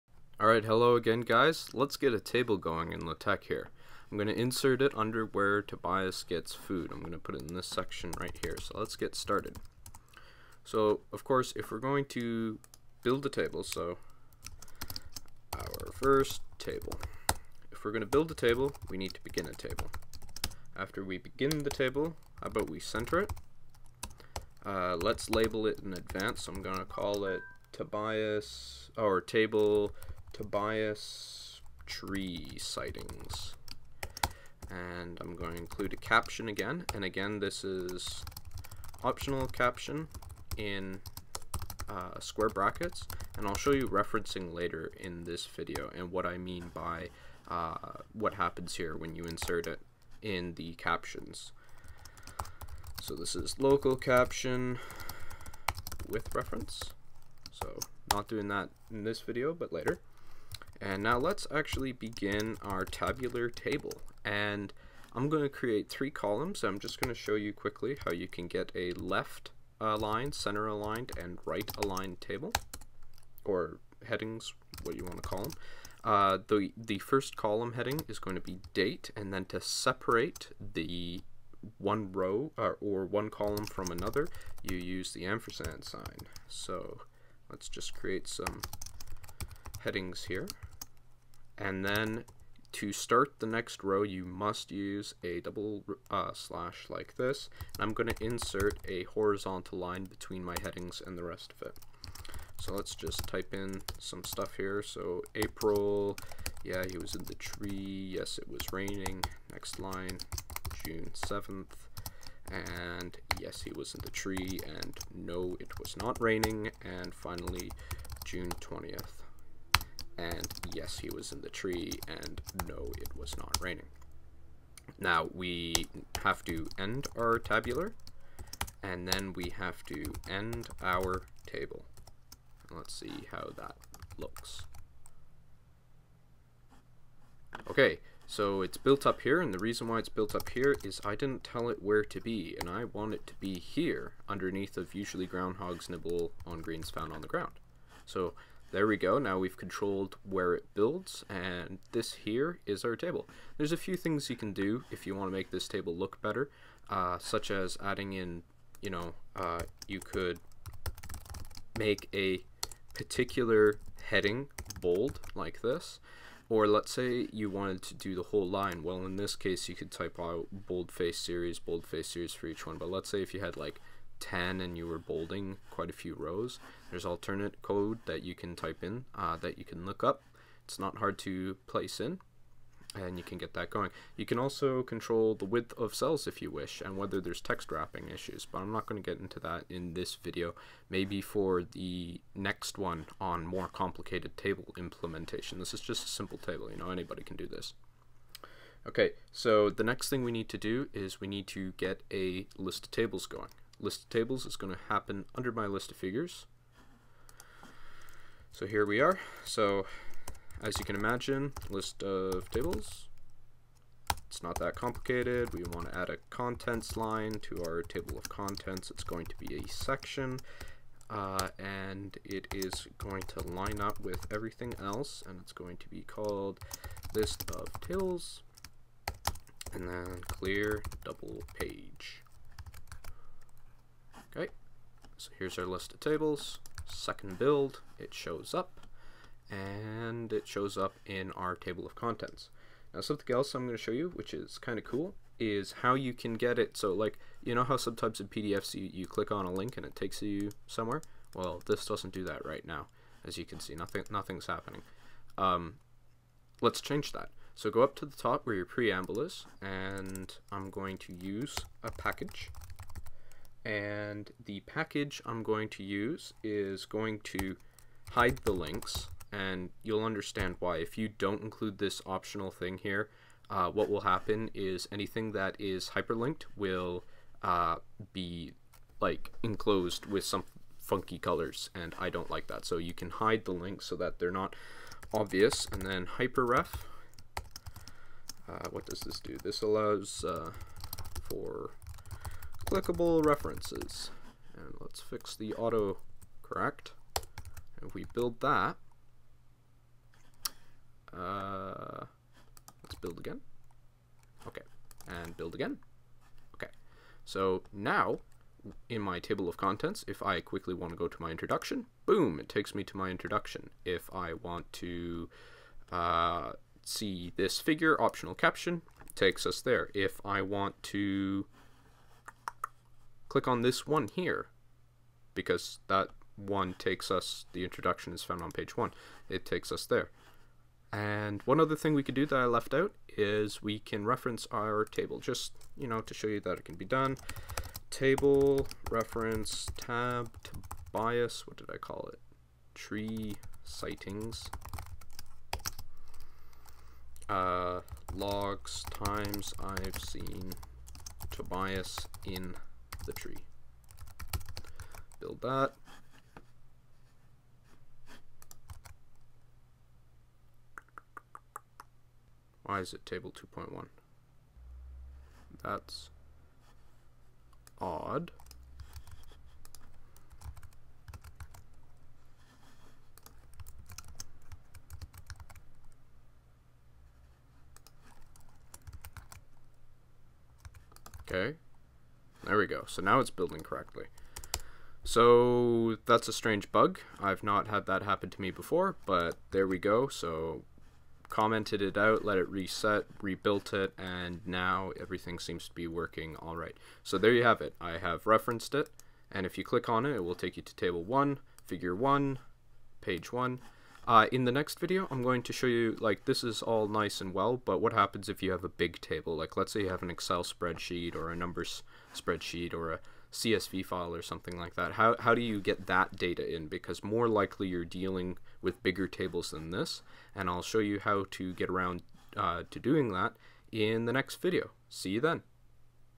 Latex Tutorial 5 of 11: Table Setup, Table of Tables
Video 5 of 11 on Latex tutorials: How to hand-bomb a simple table into your Latex report. Also how to set up a table of tables!

Next video: How to import data from a spreadsheet and how to do more complicated table layouts